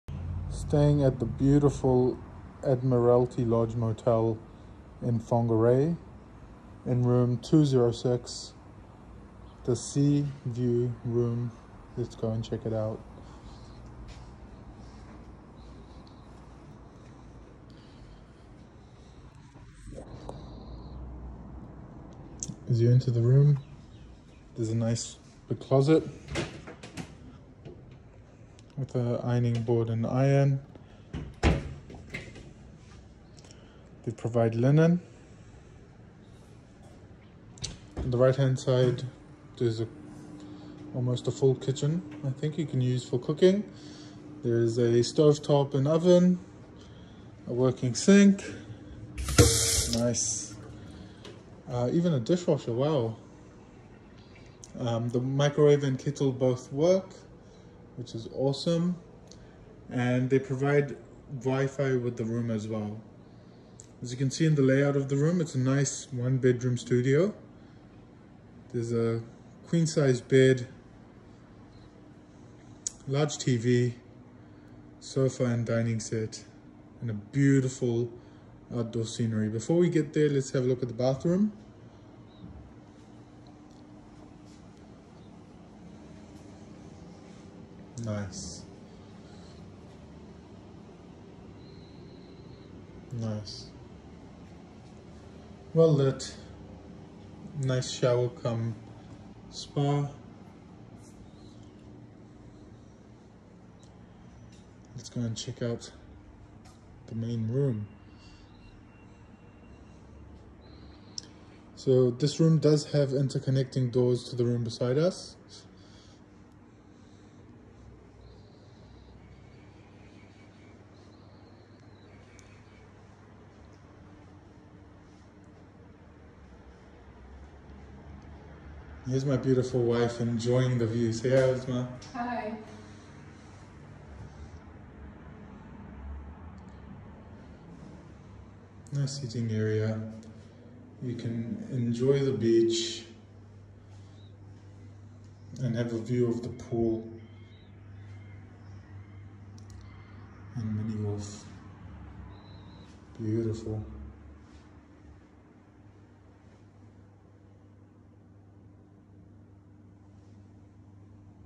Admiralty Lodge Motel, Whitianga - Sea View Room #206
We stayed at the beautiful and economical Admiralty Lodge Motel in Whitianga, NZ. This video is a showcase/review of the Sea View room which we stayed in (Room# 206)